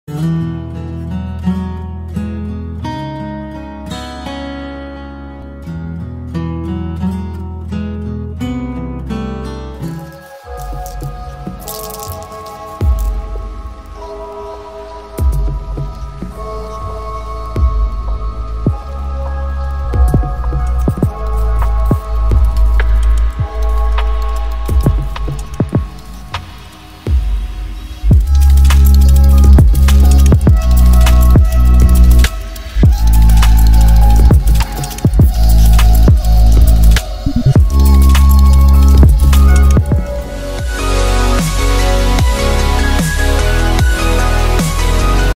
Spray Paint Art | Night Reflection Timelapse | by SenzuArt
Night Reflection timelapse spray paint art
► ADD me on TikTok: SenzuArt

See you next spray paint video!